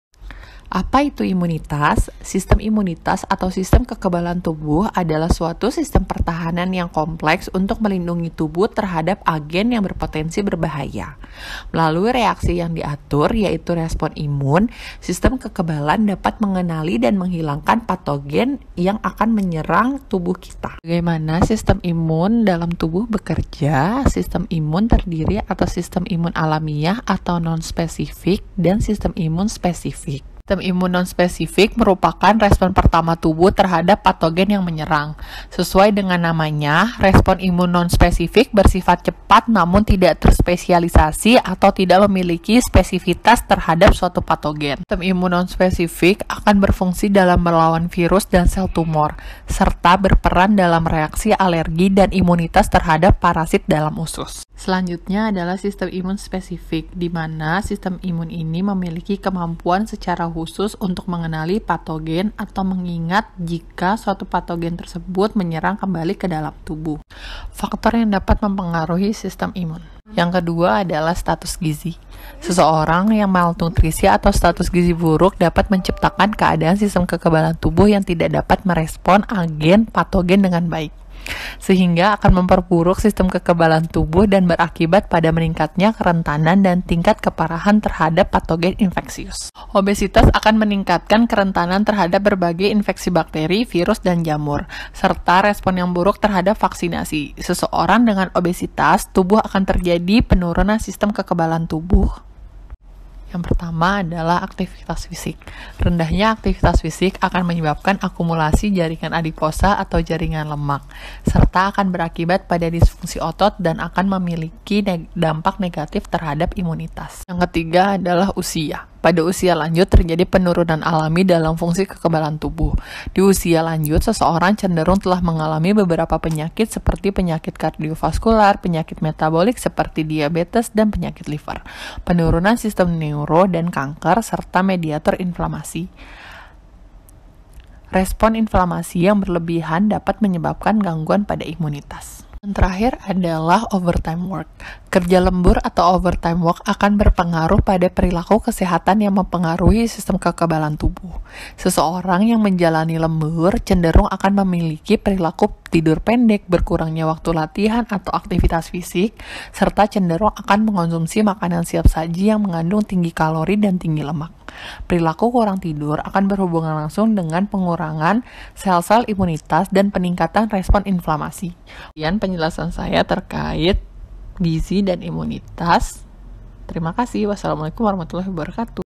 Gizi & Imunitas
Mari simak peran gizi terhadap imunitas tubuh

Oleh : Mutia Amalina Dewi, S.Gz
Pembimbing :
Ilmia Fahmi, S.Gz, Dietisien, M.Gizi